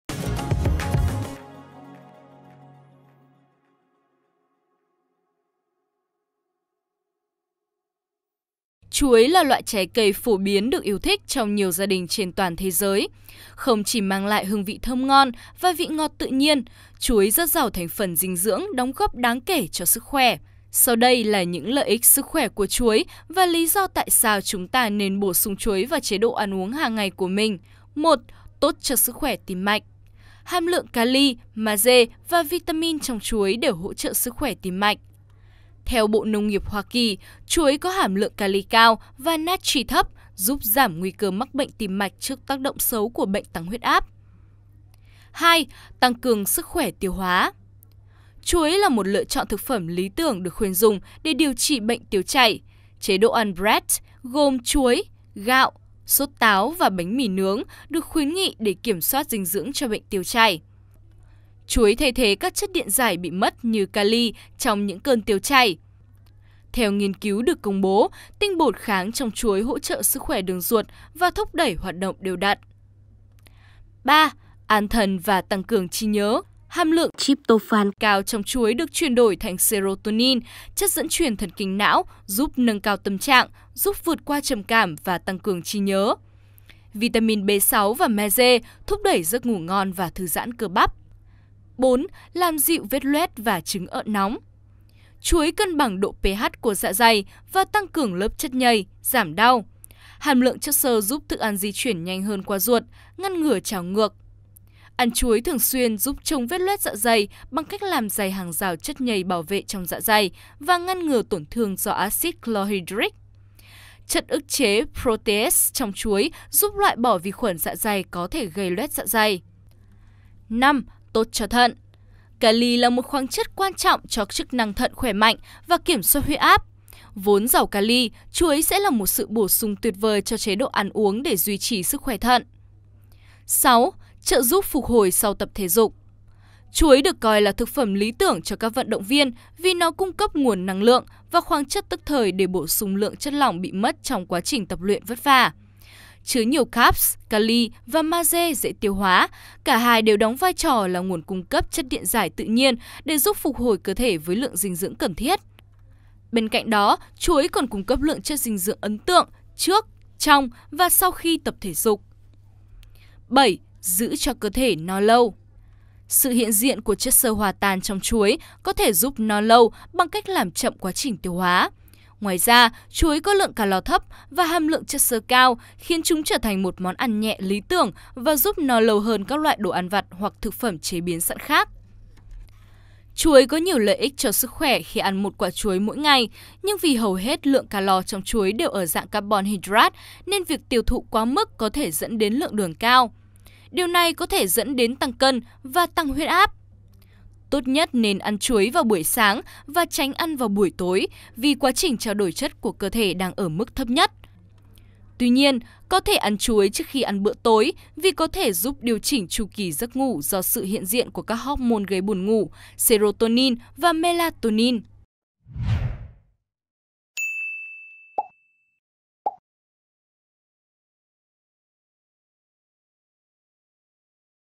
Cơ thể thay đổi tích cực khi bạn ăn chuối hằng ngày vì sao?| Toàn cảnh 24h
Toàn cảnh 24h | Chuối không chỉ cực kỳ tốt cho sức khỏe mà còn là một trong những món ăn vặt tiện lợi nhất và dễ tiêu hóa.